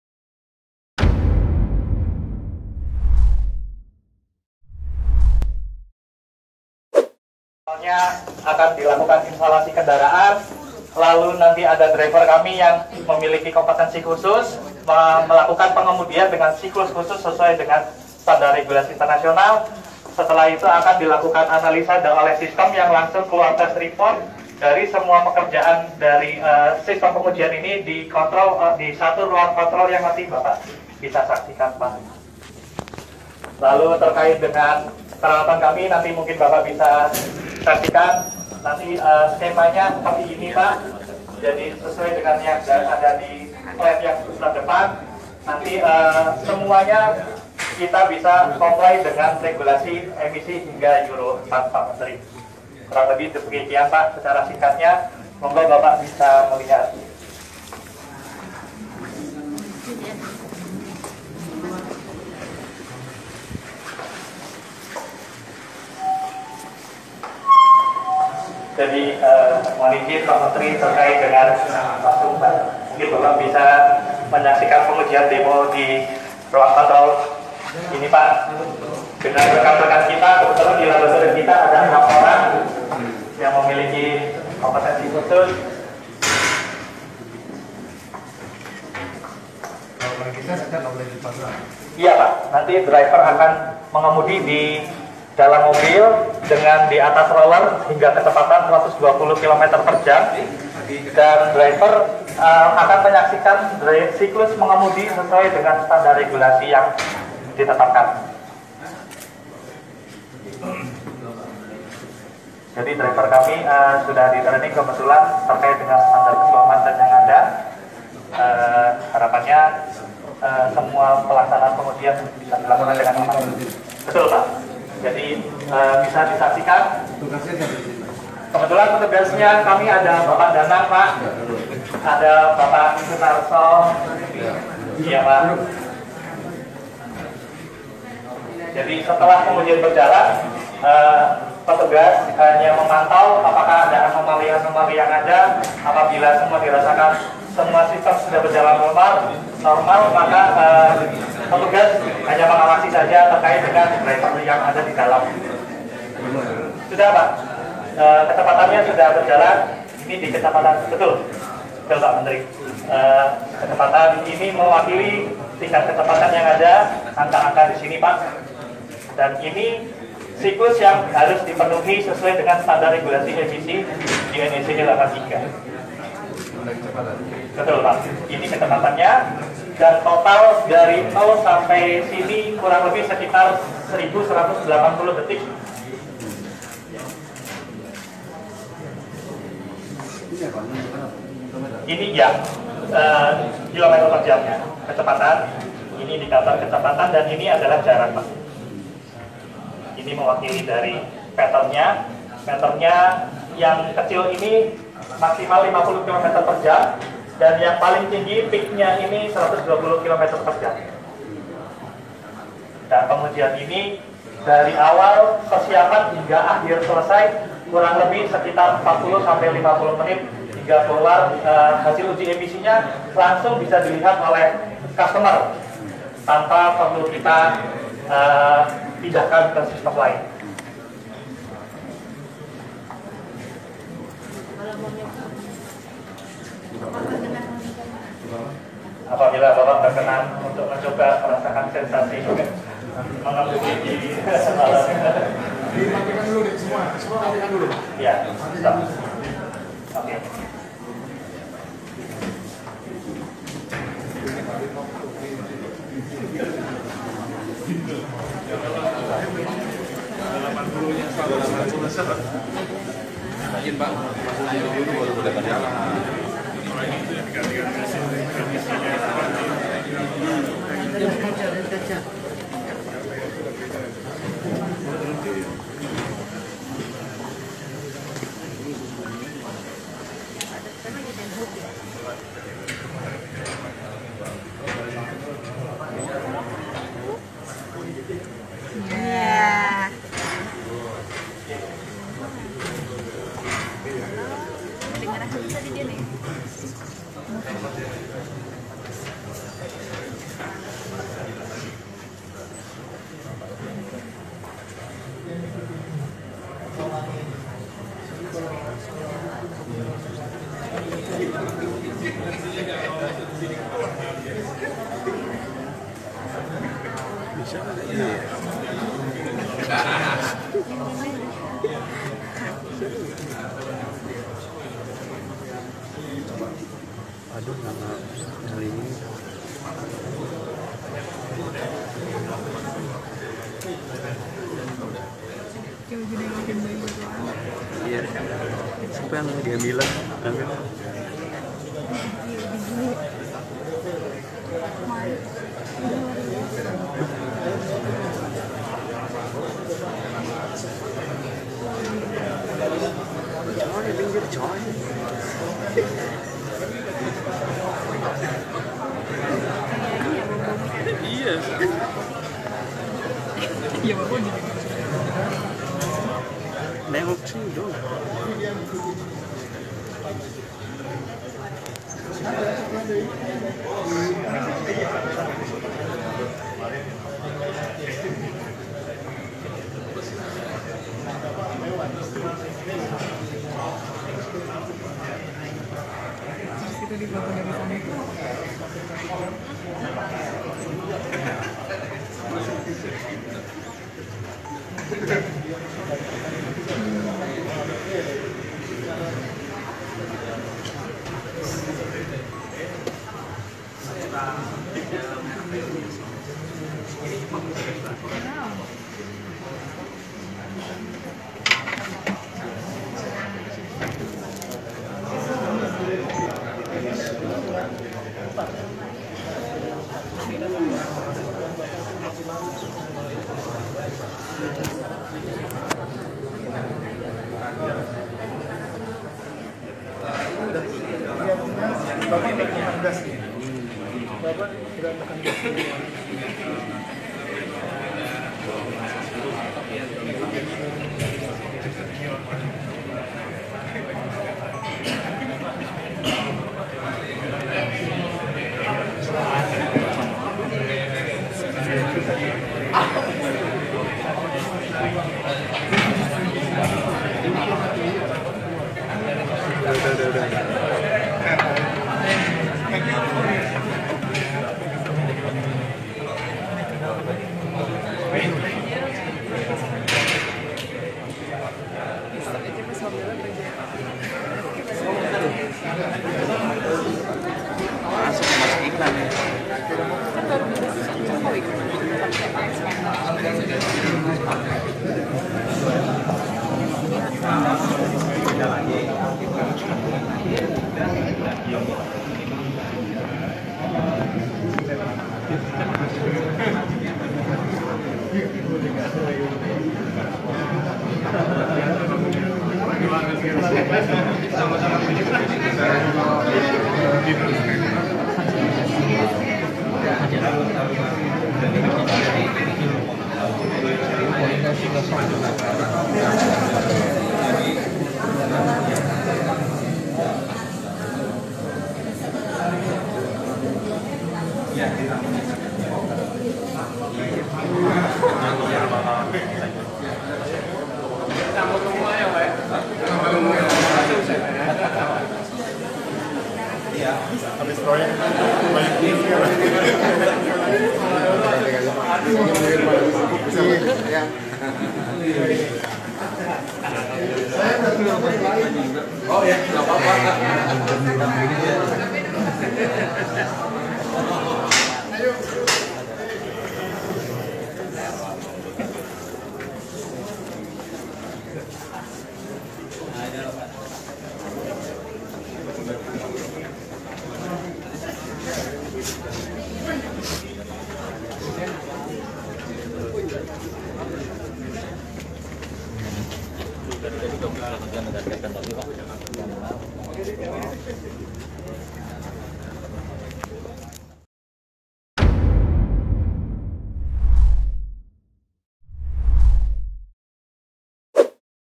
Peresmian Gedung Utama Balai Pengujian Laik Jalan dan Sertifikasi Kendaraan Bermotor (BPLJSKB)
Halo pemirsa GridOto

Kali ini reporter GridOto.com, Muhammad Adam Samudra sedang meliput peresmian gedung utama Balai Pengujian Laik Jalan dan Sertifikasi Kendaraan Bermotor (BPLJSKB).

Menteri Perhubungan Budi Karya Sumadi melakukan Peresmian Gedung Utama Balai Pengujian Laik Jalan dan Sertifikasi Kendaraan Bermotor  (BPLJSKB).

Nantinya Gedung (BPLJSKB) menjadi tempat Fasilitas Uji Emisi Sepeda Motor (R40) dan Fasilitas Uji Emisi Mobil Penumpang (R83).

Berbagai upaya pun dilakukan BPLJSKB seperti meningkatkan fasilitas sarana dan prasarana.

Dari segi sarana, BPLJSKB meningkatkan fasilitas alat uji R40 untuk sepeda motor, alat uji R83 untuk kendaraan penumpang, dan untuk kendaraan barang R49 yang nantinya mendukung uji emisi Euro4.

Dengan begitu, maka dapat menurunkan kadar emisi gas buang untuk mengurangi polusi udara loh.

Jika kamu menyukai video ini, tekan tanda jempol ke atas. Terima kasih.

Selamat menyaksikan.